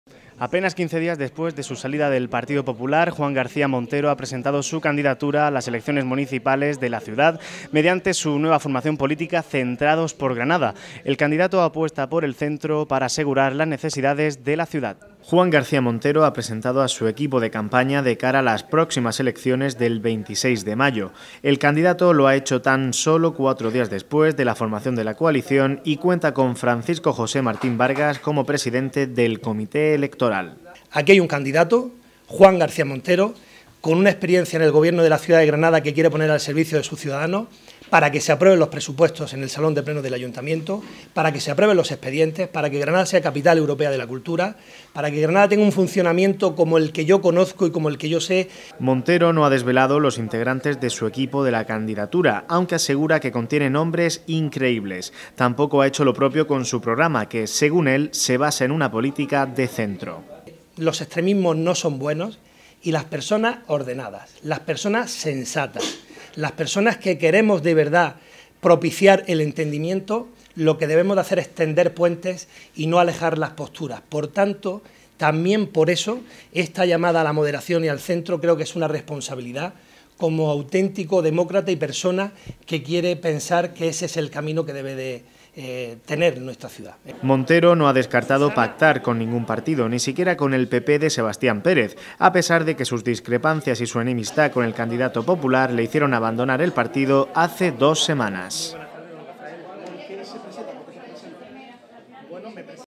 Juan García Montero ha presentado su candidatura a la alcaldía de Granada
Juan García Montero ha presentado su candidatura a la alcaldía de Granada para las próximas elecciones municipales del 26 de mayo. Tras abandonar el PP hace unas semanas, Montero se presenta por su nuevo partido: Centrados en Granada. Centrados en Granada ha dado a conocer a su equipo de campaña, pero queda pendiente de hacer lo propio con su equipo de candidatura, así como su programa electoral que, según Montero, está basado en políticas  "de centro" para "huir de los extremismos".